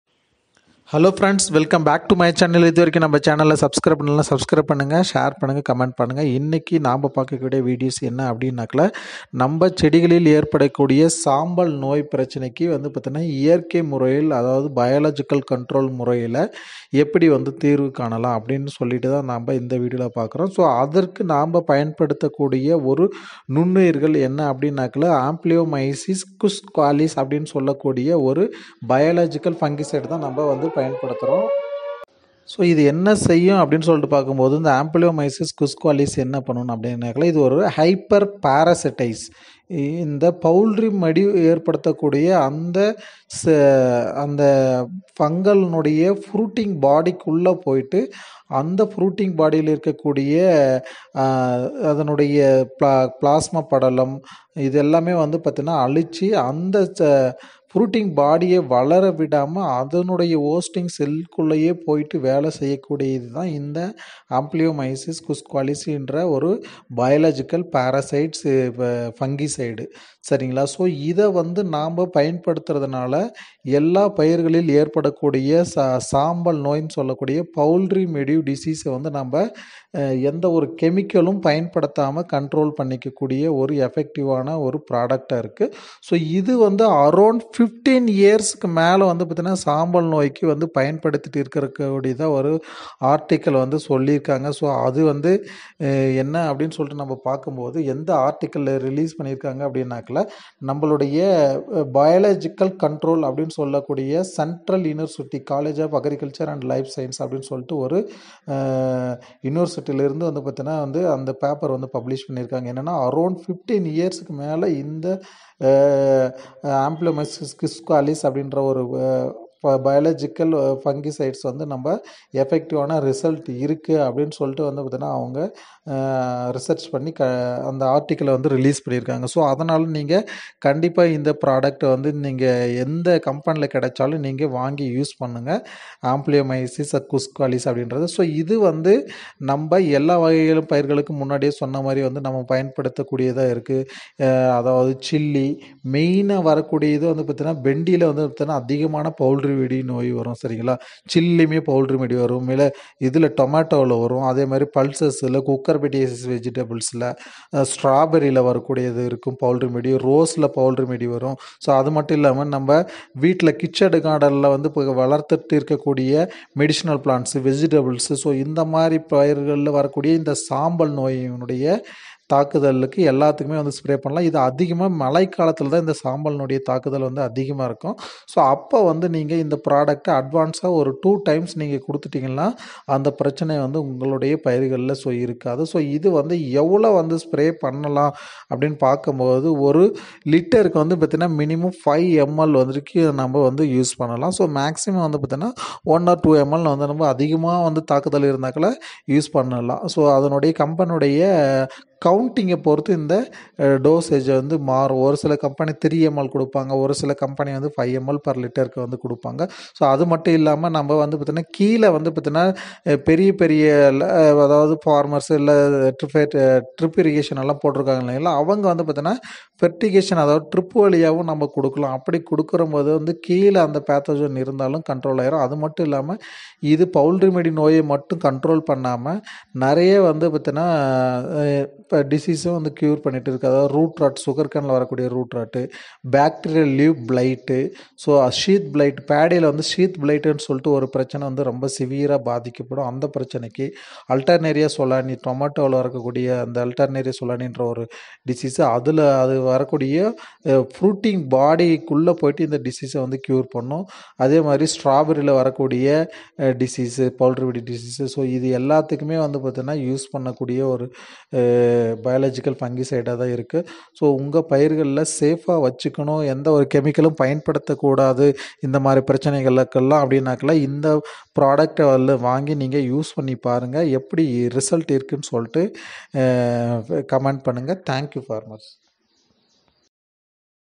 உங்கள் பயிர்களை பாதிக்கும் சாம்பல் நோய்க்கு சிறந்த தீர்வு இயற்கை முறையில் Ampelomyces quisqualis
this channel describe about seedling raising techniques and cultivation techniques
 Ampelomyces quisqualis is a hyper parasite that penetrates the pathogen and infects it by forming pycnidia (fruiting bodies) within the powdery mildew hyphae, conidiophores and cleistothecia. It grows inside the target causing degradation of the cytoplasm, leading to the collapse of hyphal strands and death.